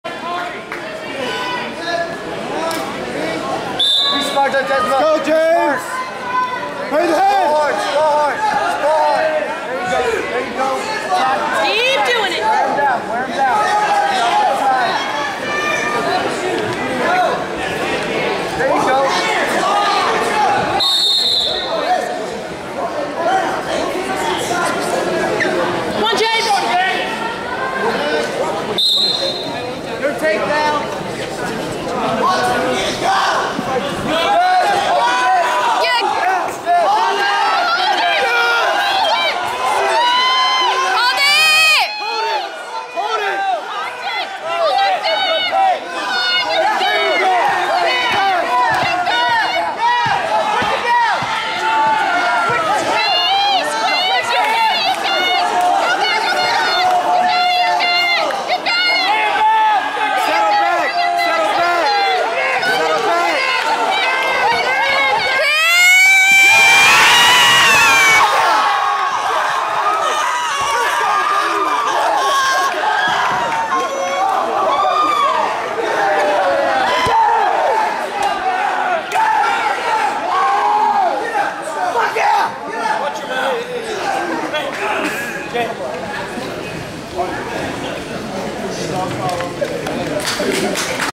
2015 regional high school wrestling tournament championship final match
Old deciding match of a regional High School wrestling match 120lbs Morton Ranch High School